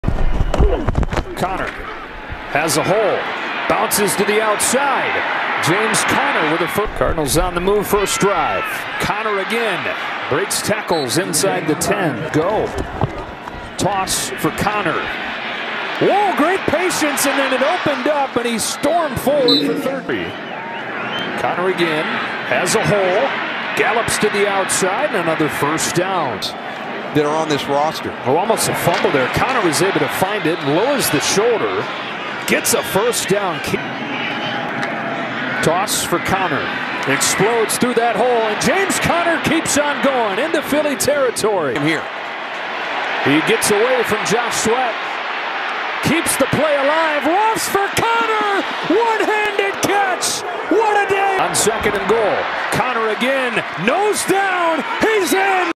JAMES CONNOR FLEW PAST THE EAGLES
James Connor had 26 carries for 128 yards and a TD vs Eagles.